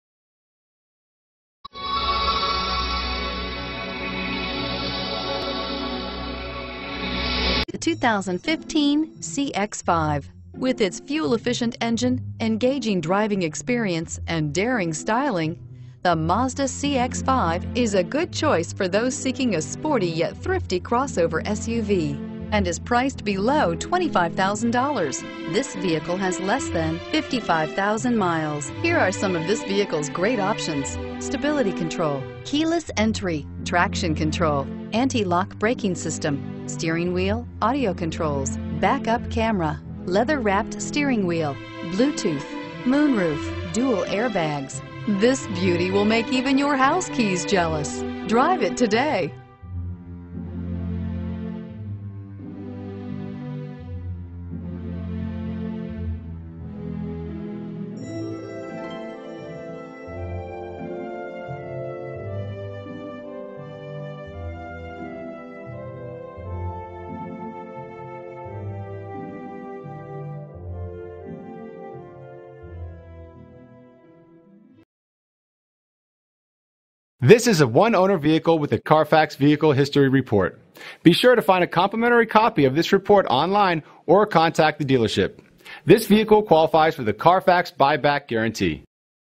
2015 Mazda CX-5 Leesburg Florida VT4174A
2015 Mazda CX-5 Grand Touring

Jenkins Volkswagen
9105 U.S. 441

Looking for the right sport utility? Today could be your lucky day. With 50663 miles, this Liquid Silver, 2015 Mazda CX-5 equipped with Automatic transmission could be yours. Request more information and set up a test drive right away.